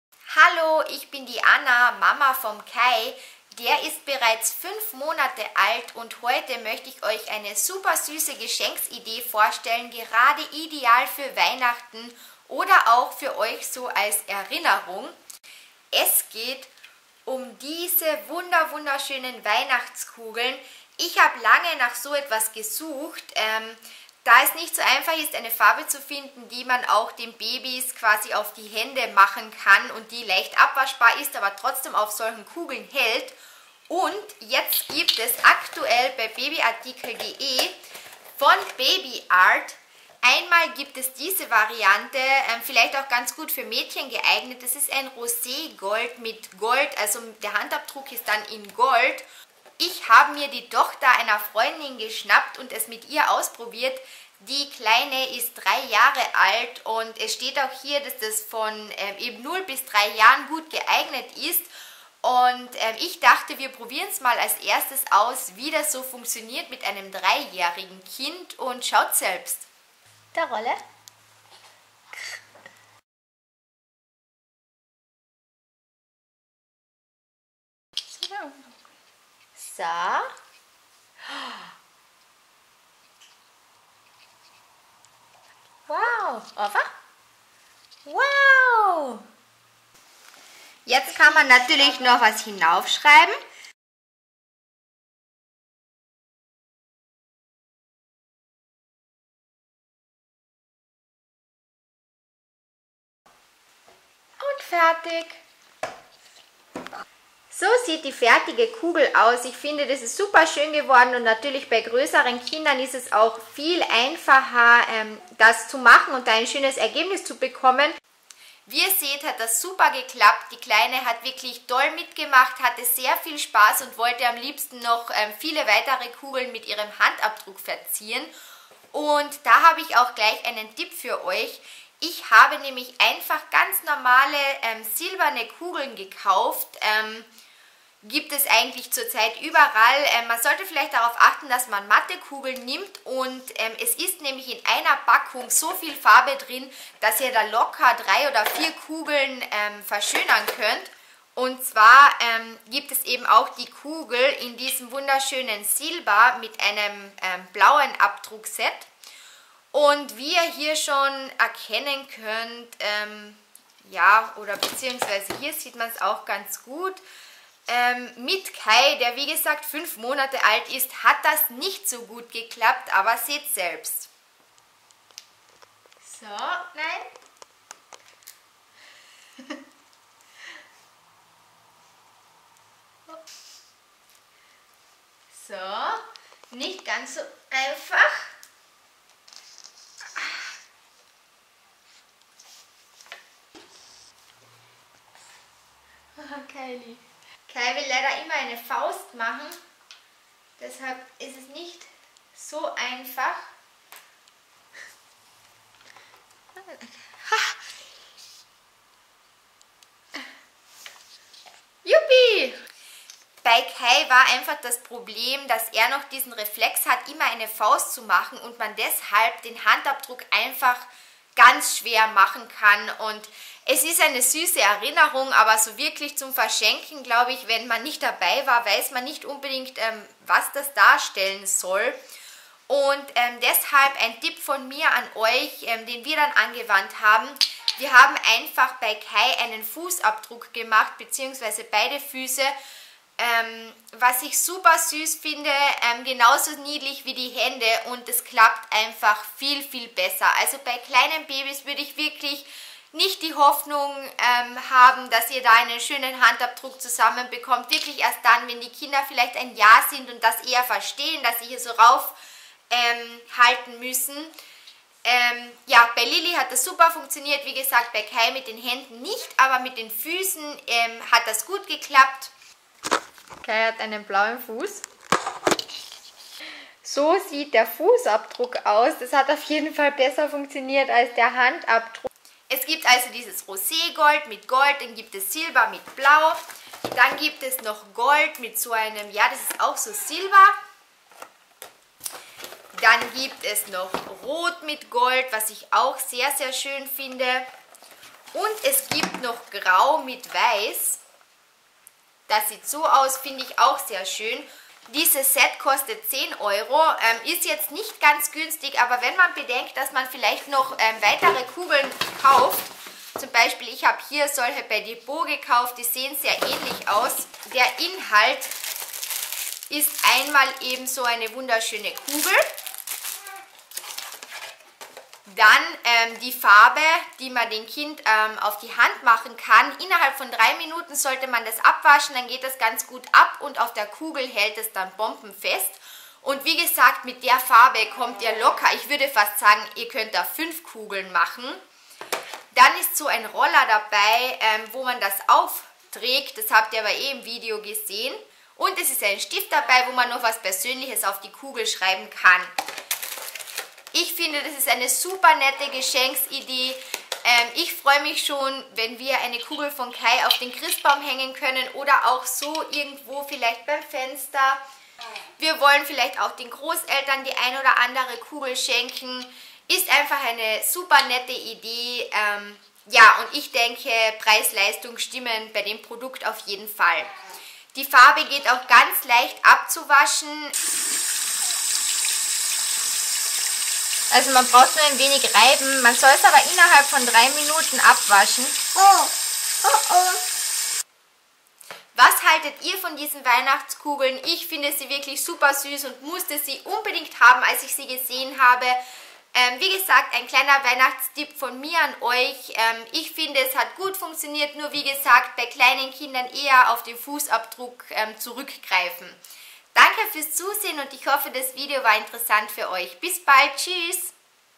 BABYART Christbaumkugeln für Abdrücke | Babyartikel.de
Heute möchte ich euch einen tollen Weihnachtsgeschenktipp vorstellen. Babyabdrücke haben sicher viele von Euch gemacht - aber auf Christbaumkugeln? Dies ist für Kinder von 0 bis 3 Jahren geeignet - wie das funktioniert erfahrt Ihr im Video...